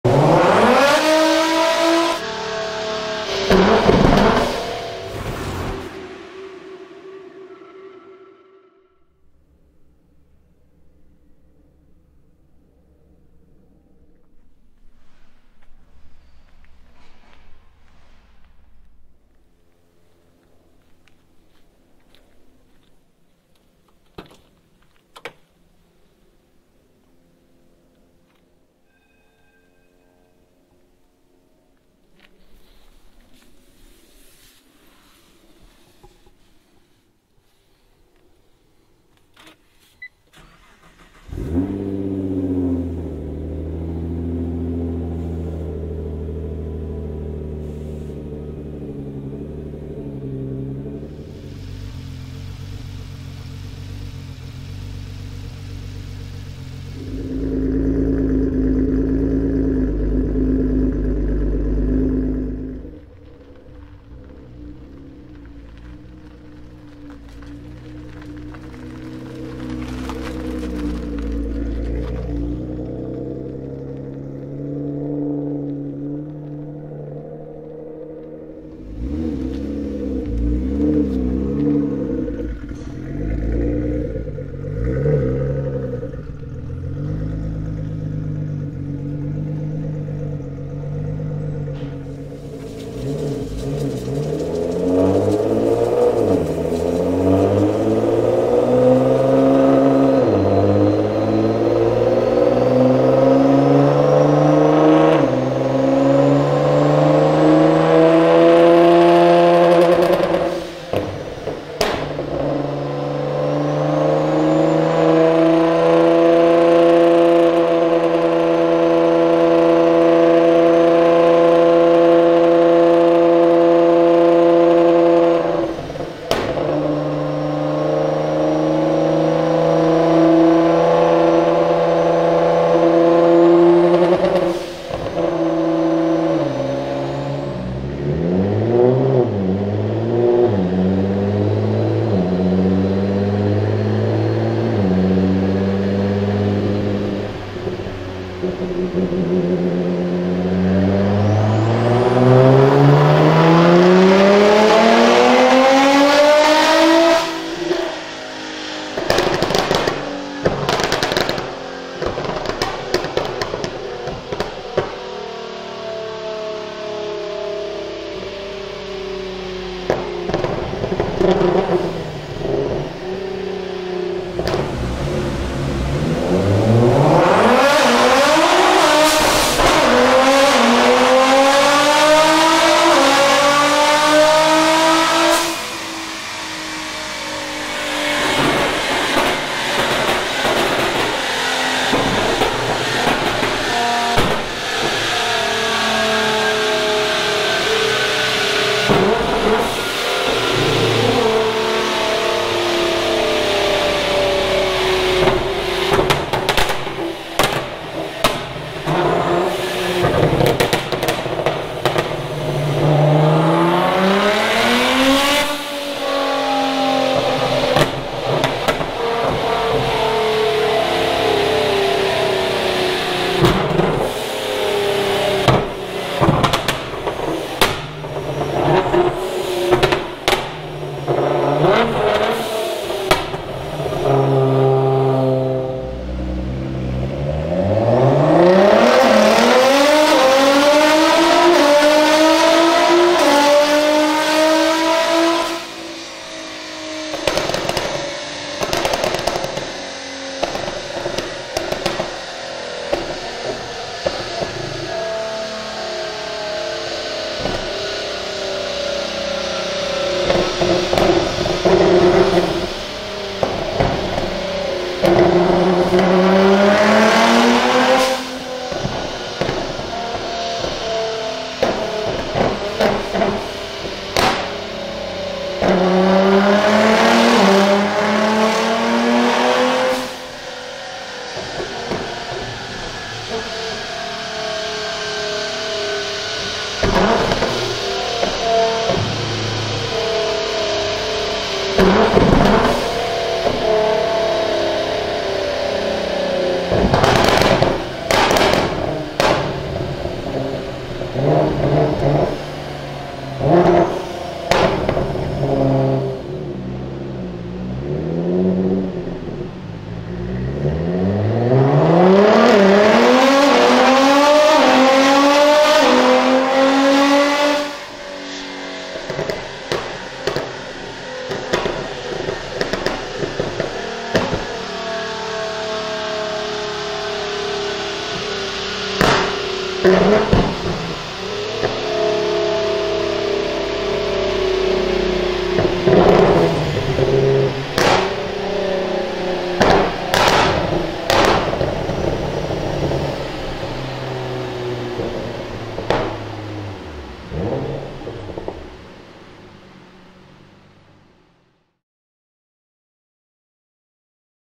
800HP Nissan GT-R FLAMETHROWER on the DYNO by VA.MA | Feat Garret G25-660 Turbos & Forged Internals🔥
● The car features forged HKS motor by Nissan Arricar, full 80mm titanium exhaust, G25-660 turbos @ 1.35bar boost and Ecutek tune by Vama Preparazioni.